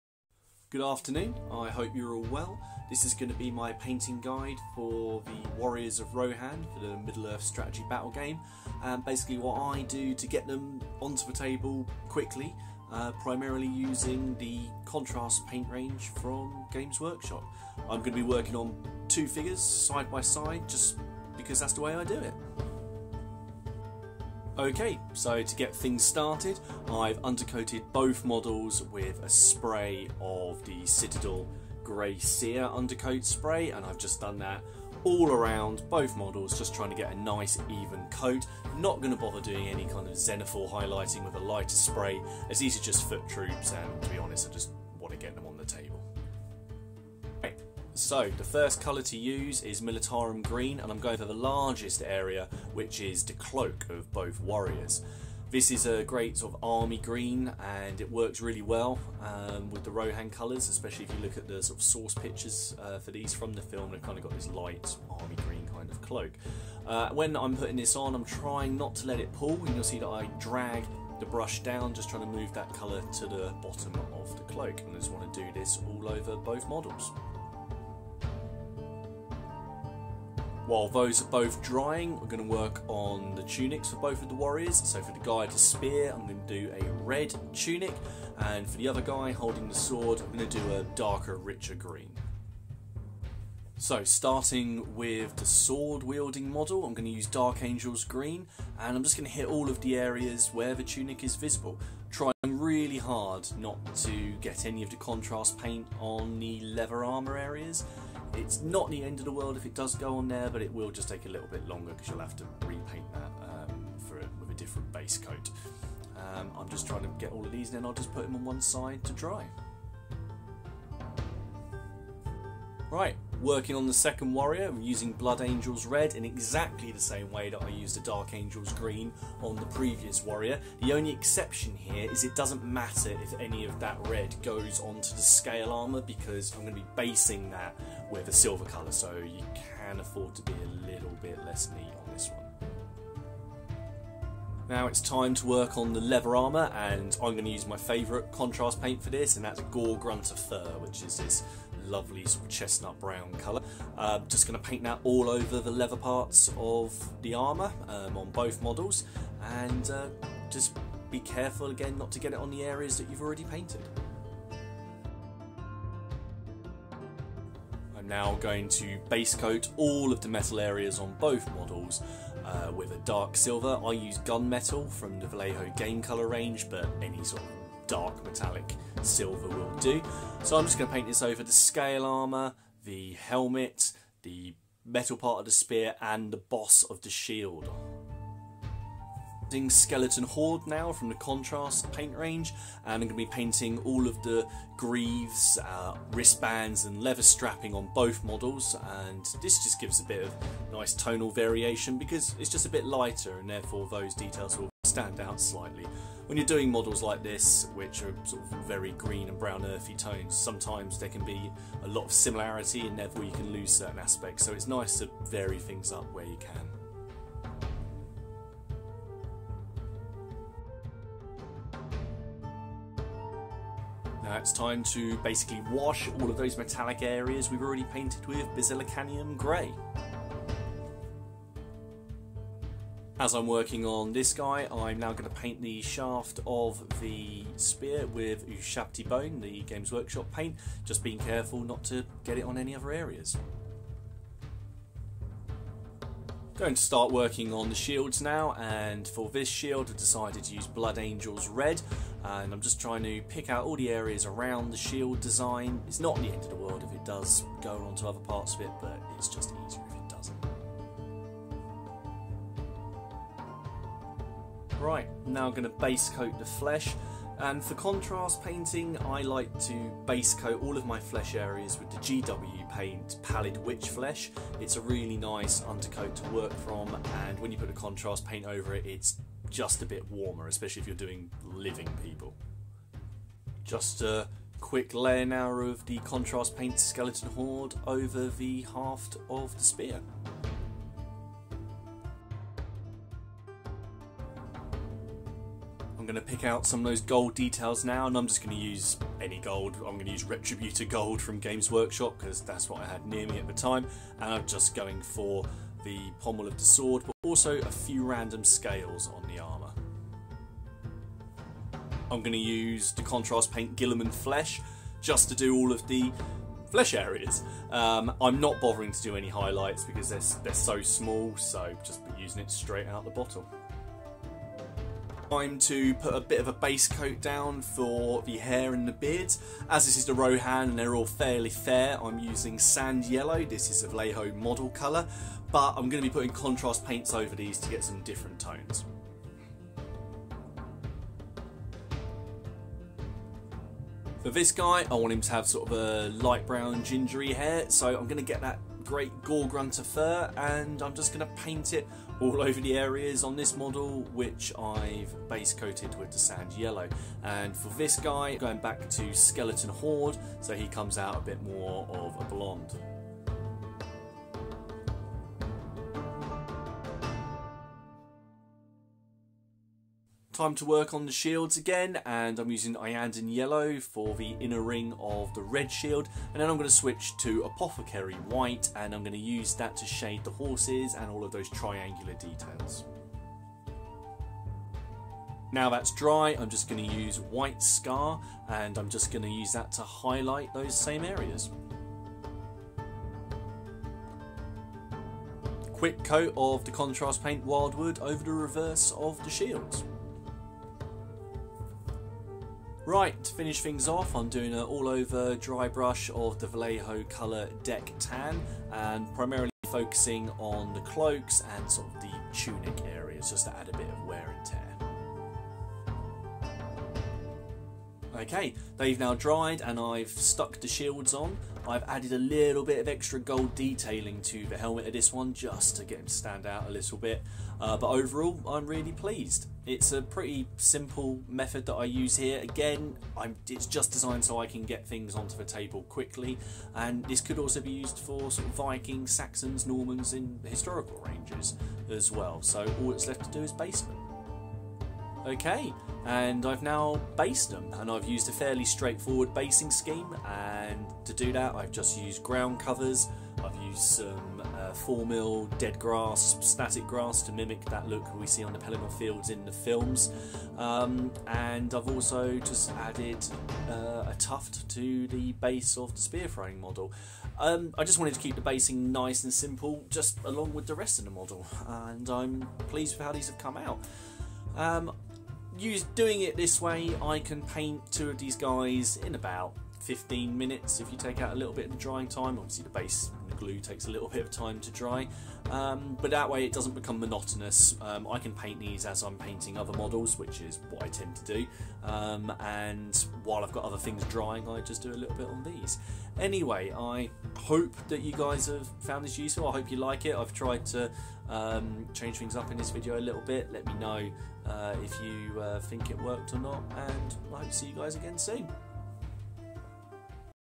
Painting - Warriors of Rohan
A quick painting video on the process i use to get my Warriors of Rohan onto the table quickly.

Paints used:

Militarum Green
Dark Angels Green
Blood Angels Red
Gore Grunta Fur
Wyldwood
Apothecary White
Iyandun Yellow
Gunmetal
Retributor Gold
Guilliman Flesh
Basilicum Grey
Ushabti Bone
Deck Tan
White Scar
Skeleton Horde